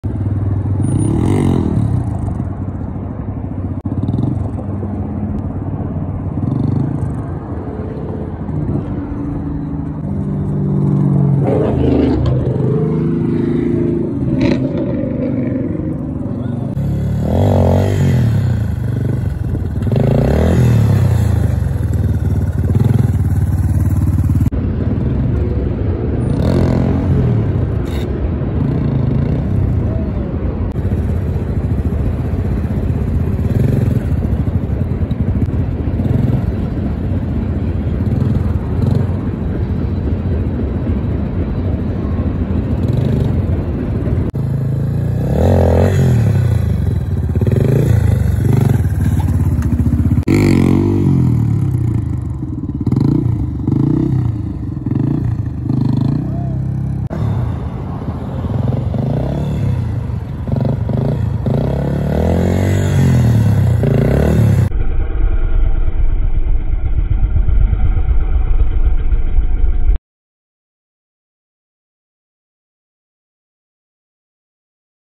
TTR230 12 OClock Wheelies !!!!!
Come watch me hit the balance point Yamaha  slow wheelies, completely badass 🔥🔥